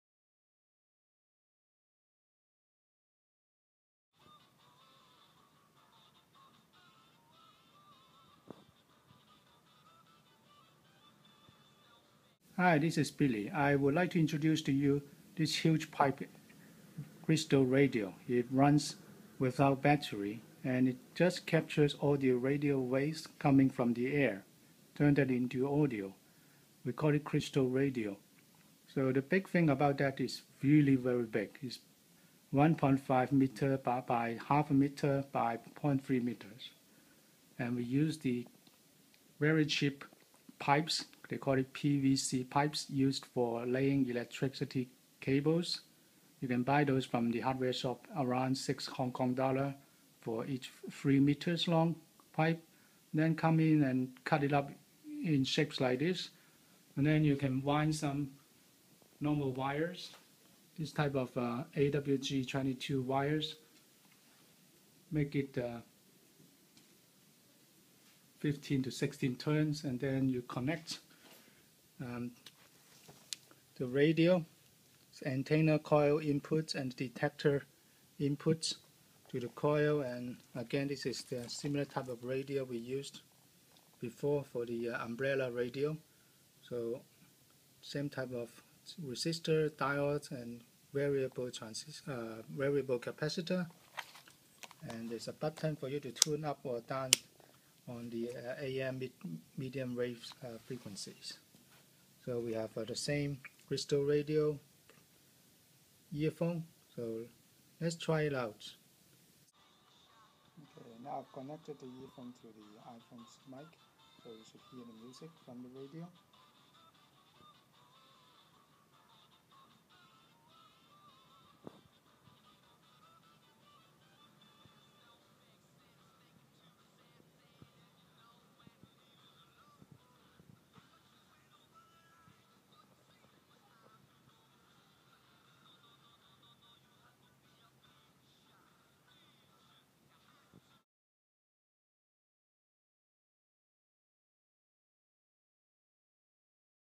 Huge Pipe Crystal Radio (Battery Free)
Create a huge pipe crystal radio using a PVC pipes as the frame for winding the coil  which doubles up as a  powerful antenna.
You can now listen to crystal radio at home without setting up 20-30M of antennas.

This radio is so strong that you don't even need to connect to the earth.
The Huge PVC Pipe  coil can double up as a super strong antenna for testing your crystal radio even at home!
Just put the huge pipe radio next to your window and point to the direction of the radio transmission station.

For full parts list, circuit diagram and instructions, visit

Billy's DIY Dream Shop
For more crystal radio projects like umbrella radio; backpack radio; hat radio, and how to make a crystal earphone for crystal radio please also